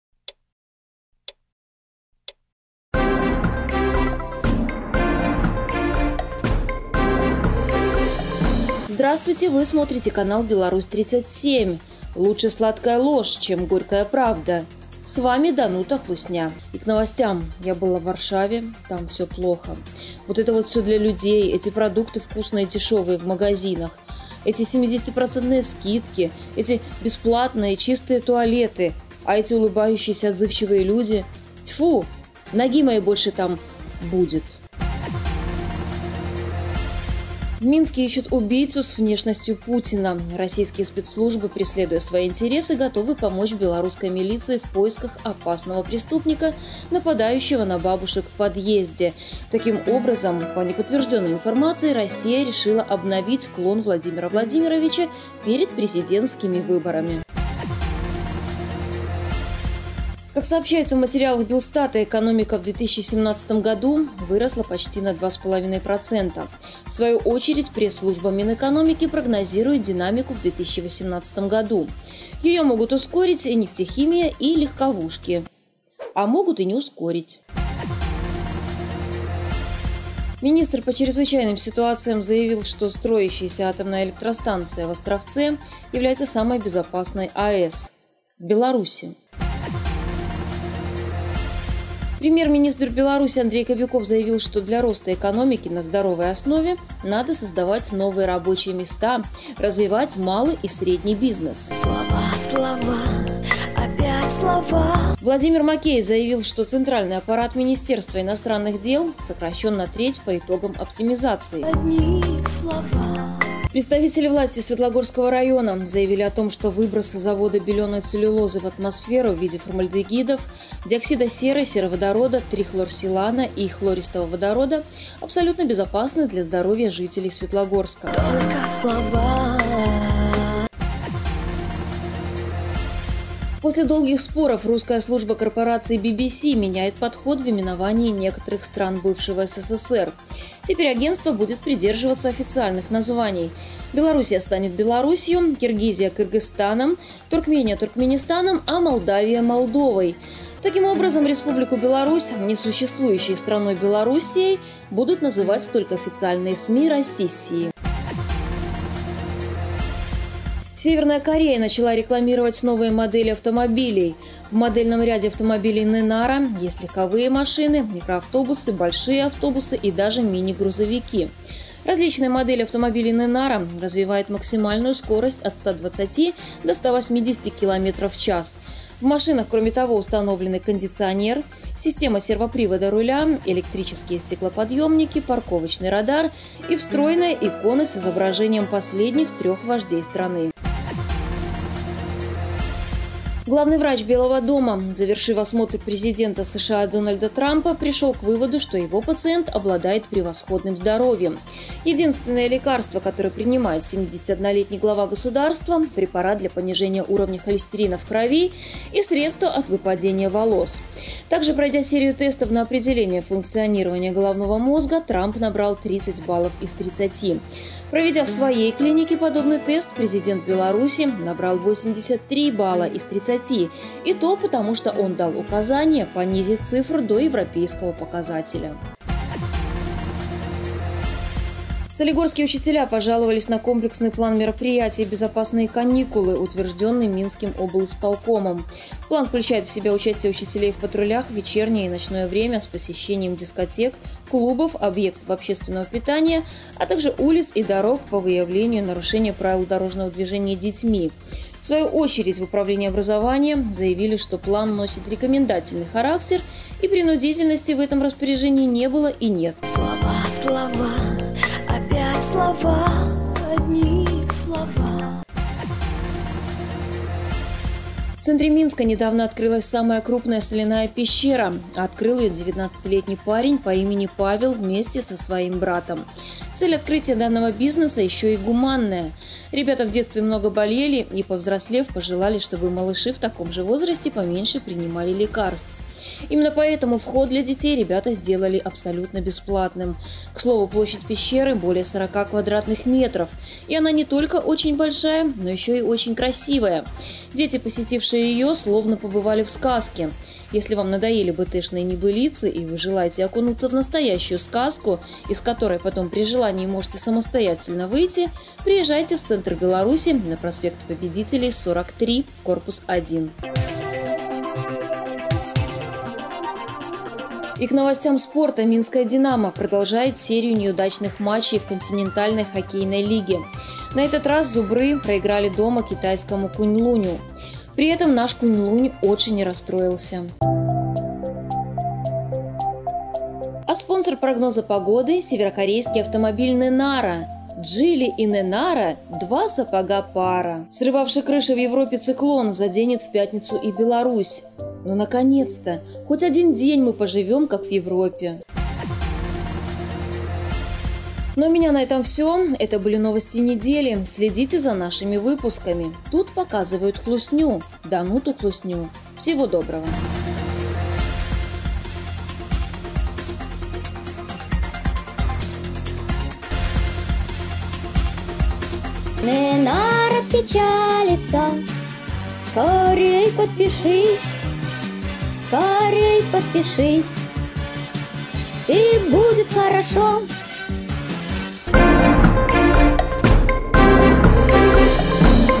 Белоруссия ИСЧЕЗНЕТ. Лукашенко VS Трамп. Главные новости Беларуси. ПАРОДИЯ. Выпуск 22
Пародия на реальные новостные выпуски государственных телеканалов Беларуси - БТ, ОНТ и региональные телеканалы. Главные новости Беларуси - это ПАРОДИЯ! Беларусь 37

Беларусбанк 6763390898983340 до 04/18

Какие последние новости сегодня в выпуске у Дануты Хлусни?.. Точнее, главные новости:
В Минске ищут убийцу с внешностью Путина, Россия решила обновить клон Владимира Владимировича;
19-летний парень открыл соляную пещеру в центре Минска на пр. Победителей, 43 к.1 Стоимость можно посмотреть по ссылке
Белстат, Минэкономики говорят об экономическом росте;
Премьер-министр Кобяков - надо развивать бизнес;
МЧС - АЭС в Островце самая безопасная;
Власти заявили, что выбросы завода белёной целлюлозы в Светлогорске безвредны;
Белоруссия исчезнет с экранов Русской службы ВВС;
Северная Корея (КНДР) рекламирует новые автомобили "Нэнара";
Врач Дональда Трампа заявил о здоровье Президента США, но Лукашенко здоровее;
Солигорские учителя не хотят патрулировать улицы и дискотеки;
Новости спорта, прогноз погоды;
И о другом с юмором и приколами...
22 выпуск